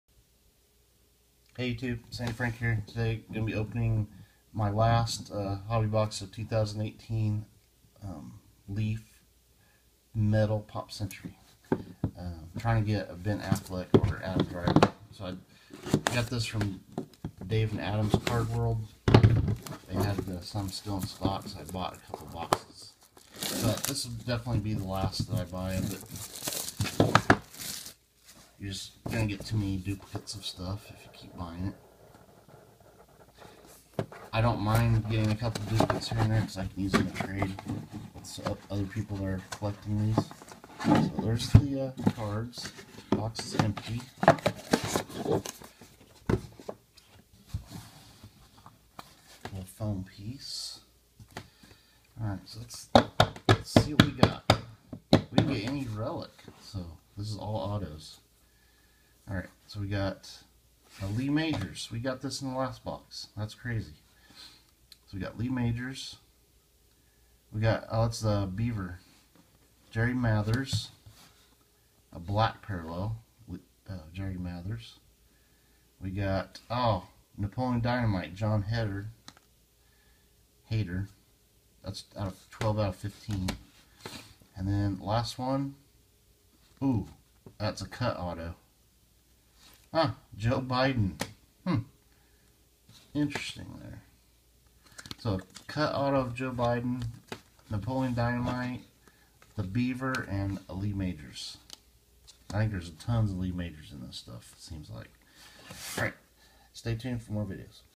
Opening One Last Hobby Box of 2018 Leaf Pop Century Metal Cards - CUT AUTO!!
Entertainment Card Pack Openings